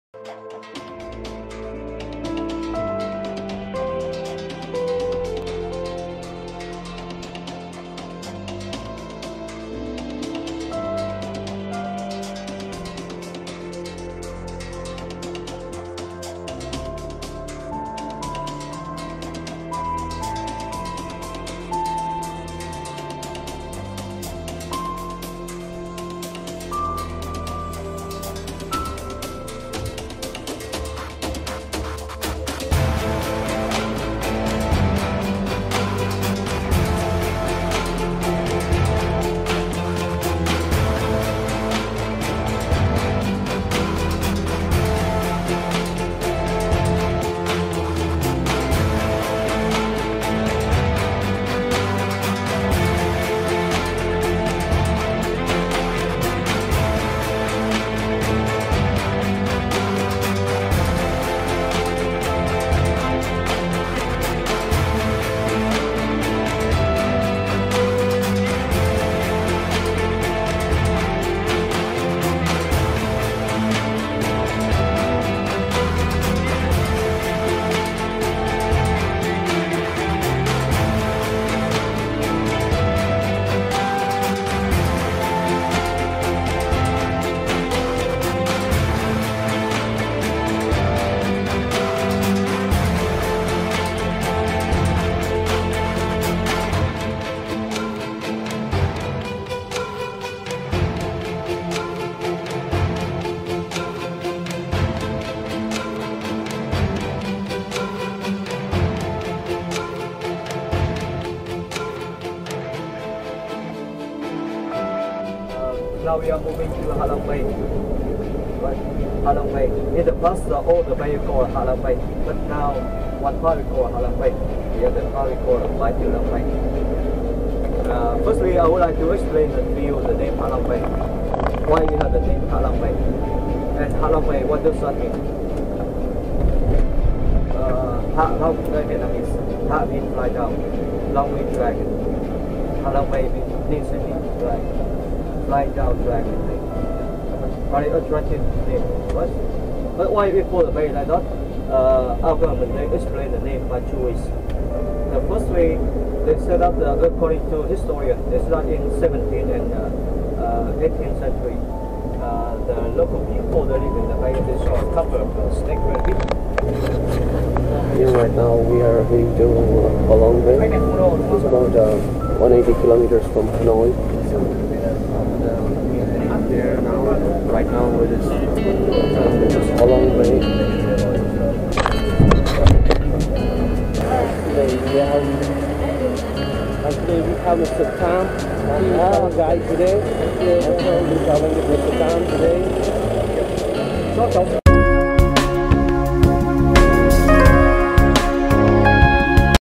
HANOI TO HALONG BAY PIER by Road || Vietnam
Travel distance to Ha Long Bay is about 180 km by land from Hanoi and travel by bus to Ha Long takes about 3,5-4,5 hours. You can travel by direct bus to Ha Long or by train from Hanoi to Hai Phong (40 km from the town of Ha Long).

The shuttle bus can be understood as “tourist bus”, leaving Hanoi around 08:00 and reach Halong Pier at 12:00 .